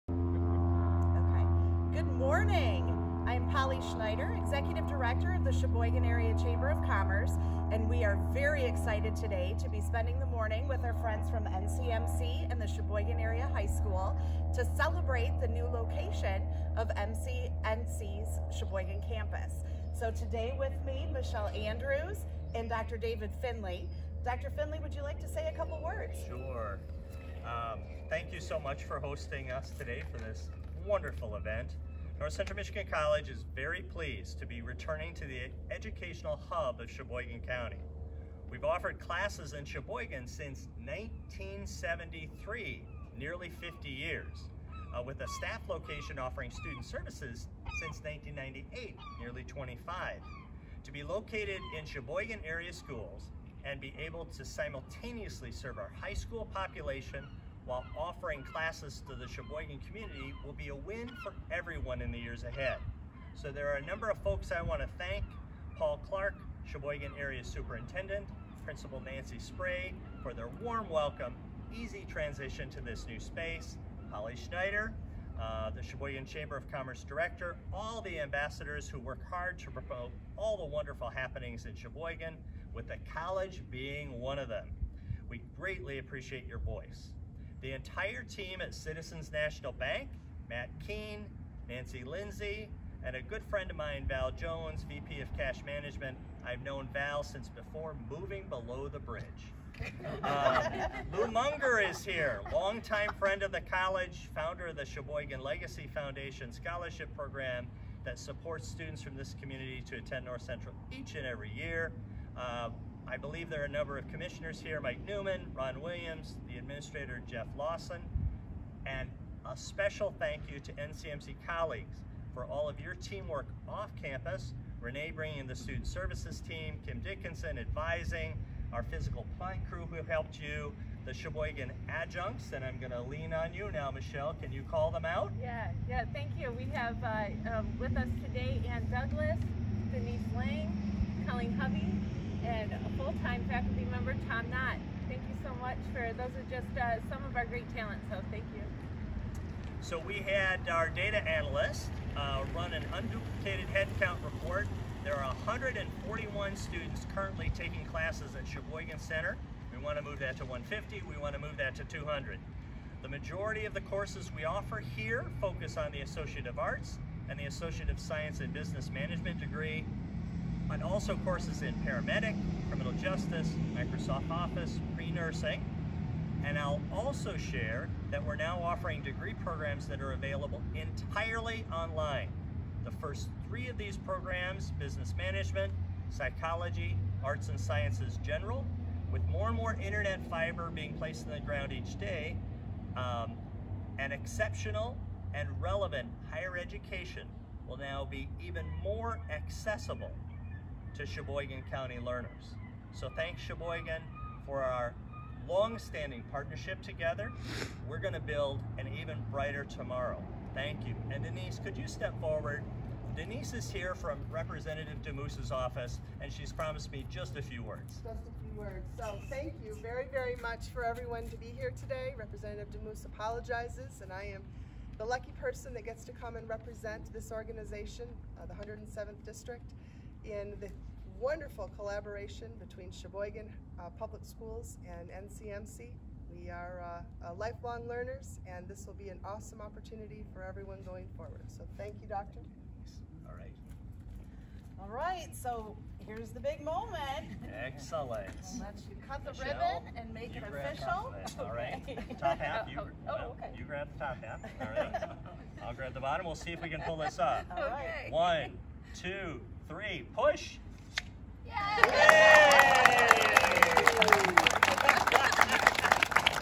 CACC Ambassadors Celebrate NCMC's New Location at Cheboygan High School with a Ribbon Cutting
Cheboygan Area Chamber of Commerce Ambassadors and Staff helped North Central Michigan College celebrate their new location at the Cheboygan High School with a wonderful gathering and ribbon cutting!